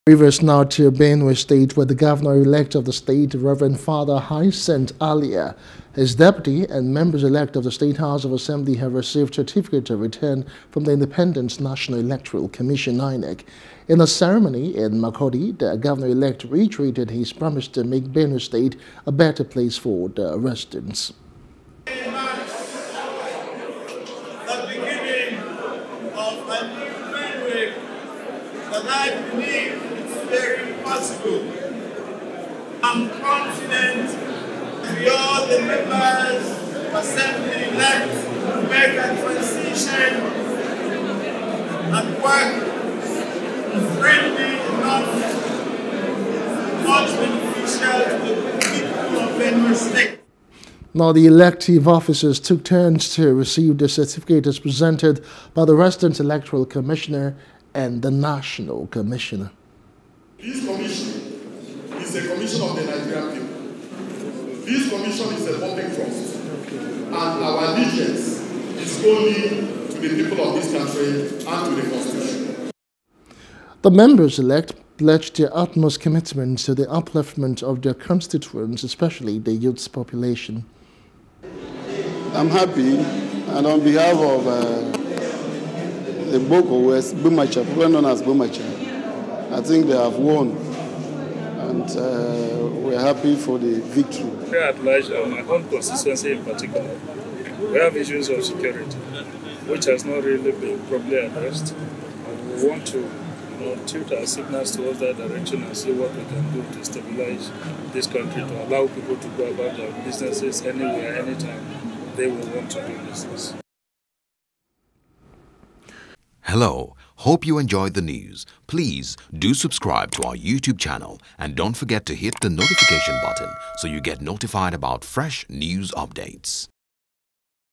Benue Elections: Governor-Elect, Members-Elect Receive Certificate Of Return
The governor-elect of Benue State, Rev. Fr. Hyacinth Alia, his deputy, and members elect of the State House of Assembly have received certificates of return from the Independent Electoral Commission, INEC.